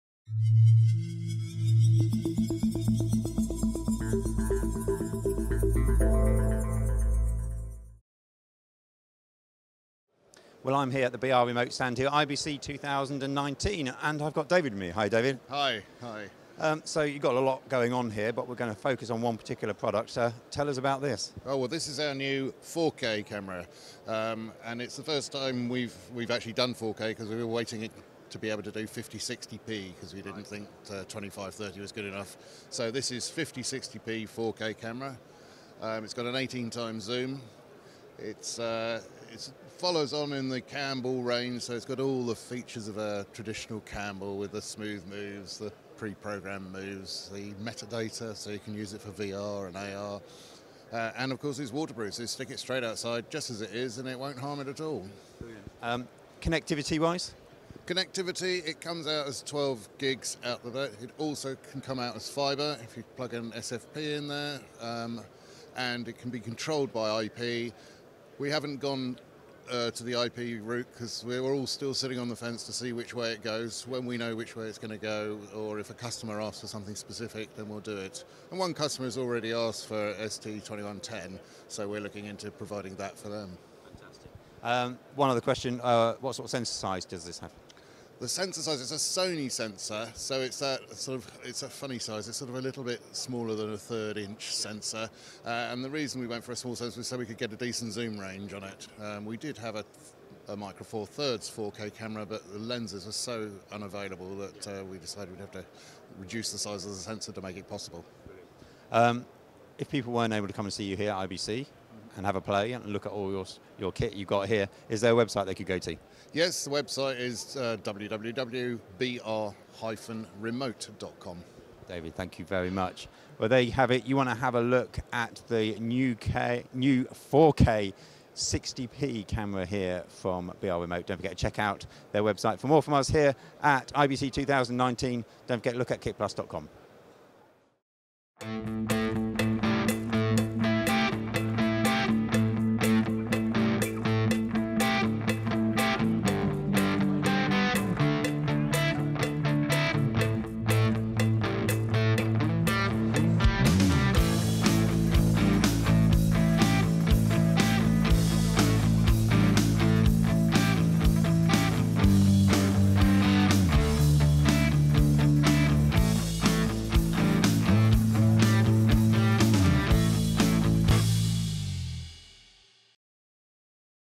CamBall 4Ki 50/60p 18x Zoom waterproof 12g SDI PTZ camera from BR Remote at IBC 2019
We see what's new at BR Remote. Direct from IBC 2019, Amsterdam